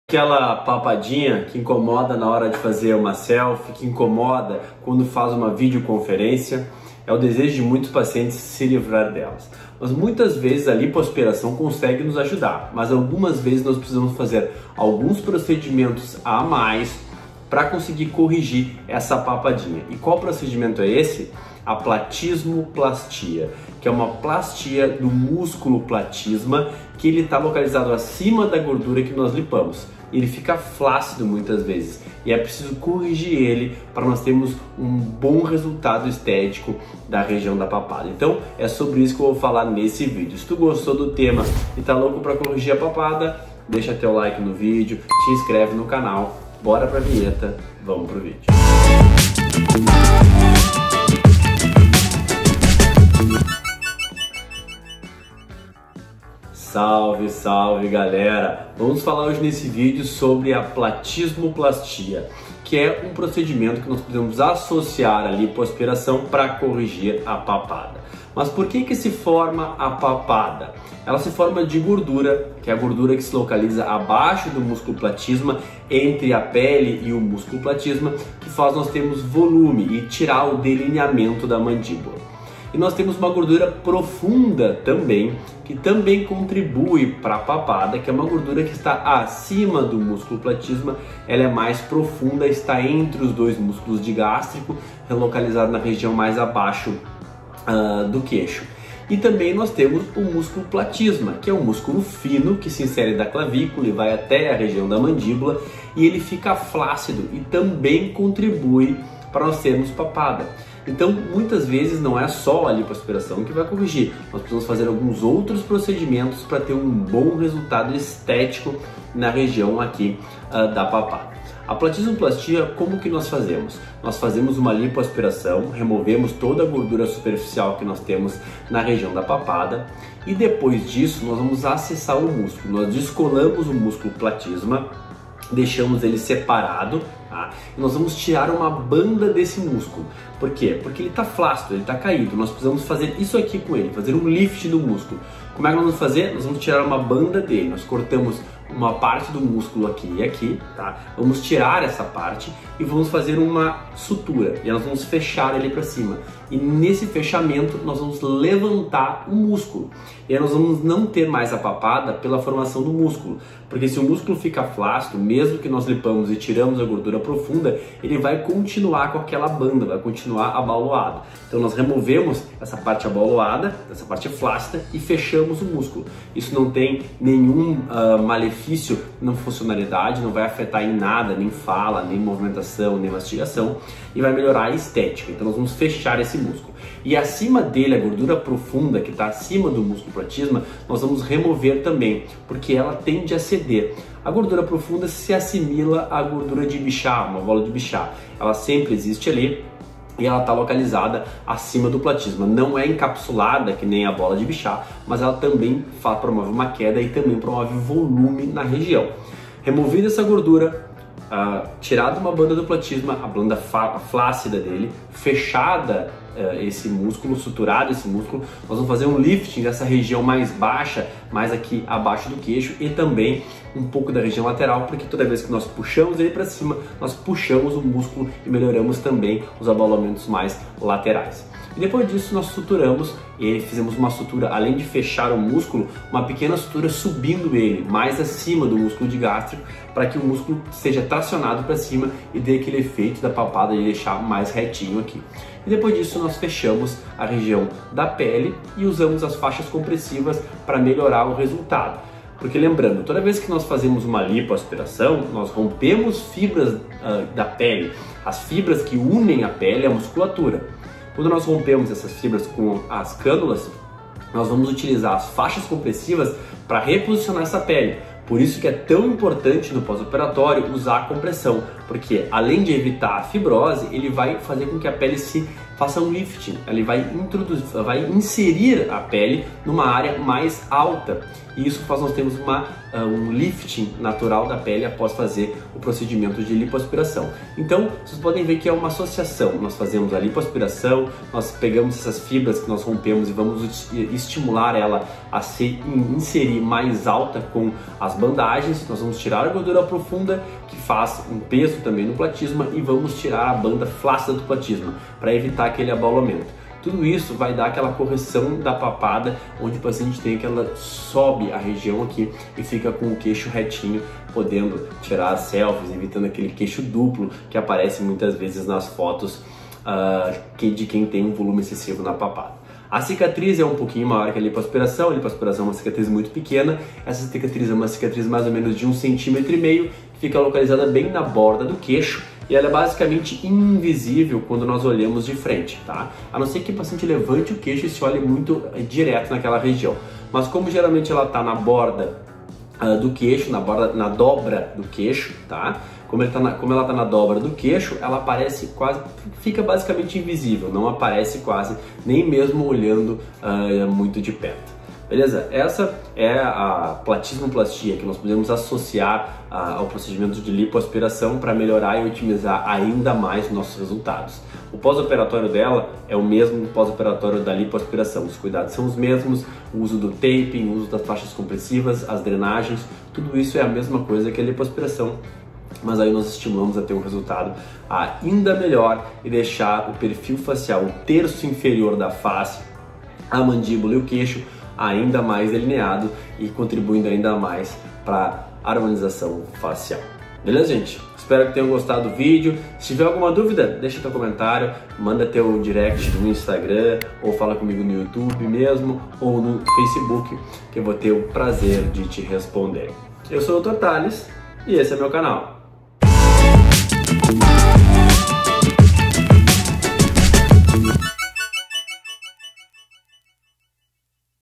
Correção da PAPADA e da FLACIDEZ do pescoço - Plastismoplastia.
Sabe aquele momento de tirar a selfie que a papada atrapalha? ou quando vamos gravar um video ou participar de uma reunião online ?  A PlatismoPlastia ou cervicoplastia  corrige a flacidez provocada pelo músculo flácido, orientando o lifting da pele do pescoço. Vem comigo para entender esse procedimento passo ma passo.